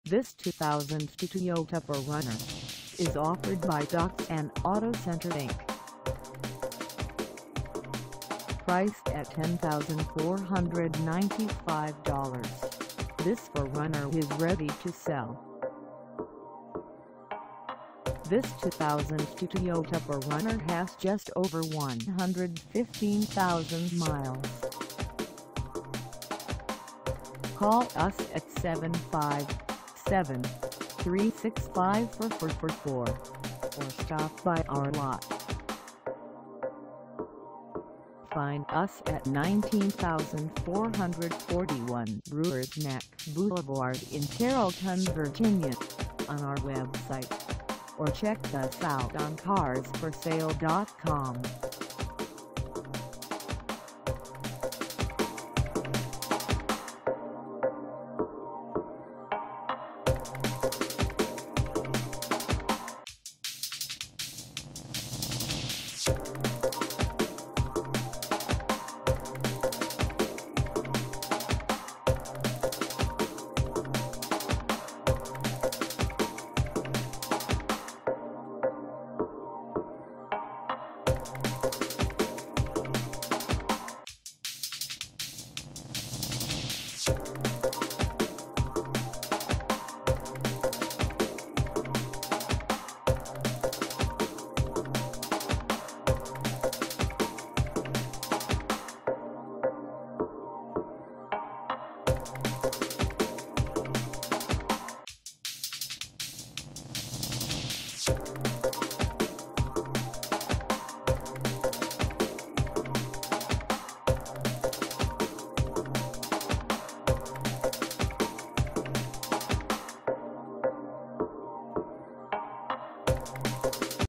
2002 Toyota 4Runner Limited 4WD for sale in Carrollton, VA 2
This 2002 Toyota 4Runner Limited 4WD is for sale in Carrollton, VA 23314 at Dotson Auto Center Inc..
Engine: 6-Cylinder V6, 3.4L; DOHC 24V
Transmission: 4 Speed Automatic
Color: champain
Mileage: 115000
Contact Dotson Auto Center Inc. 19441 Brewers Neck Blvd. Carrollton, VA 23314 to test drive this 2002 Toyota 4Runner Limited 4WD today.